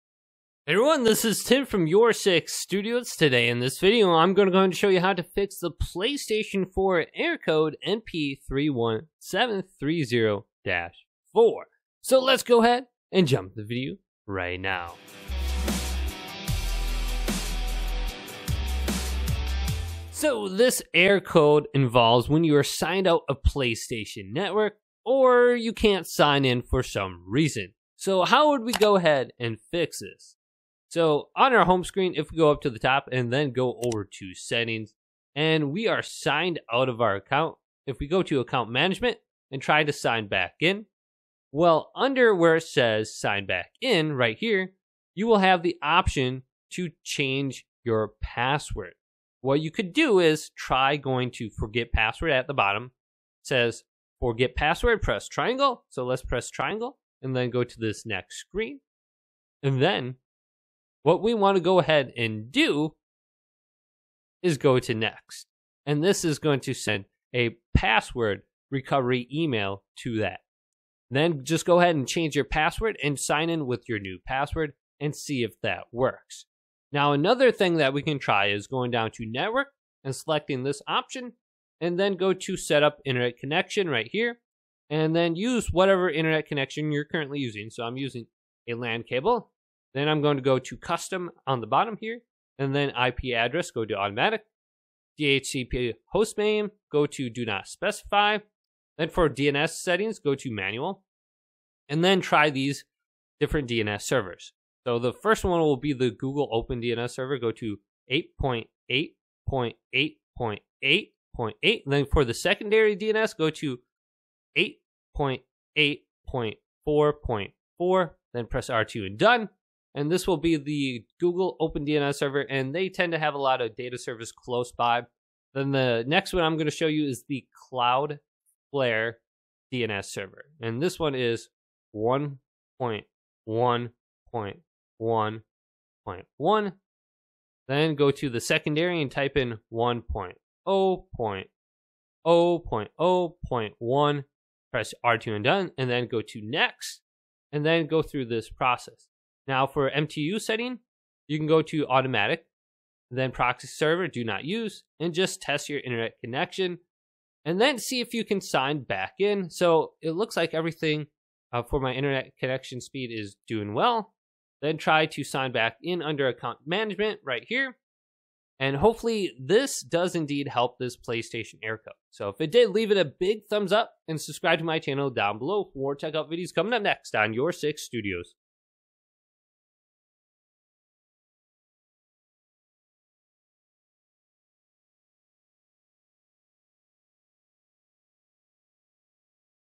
How to Fix Can't Sign into PS4 Account (Error Code NP-31730-4)
Do you want to know how to FIX Can't Sign into PS4 account and fix the error code NP-31730-4. To do this, go to settings, then go to users and account and sign in. At the bottom select forget password and try resetting your password and then log back into. Then go to network and setup a new connection and change the DNS server and test your connection. ...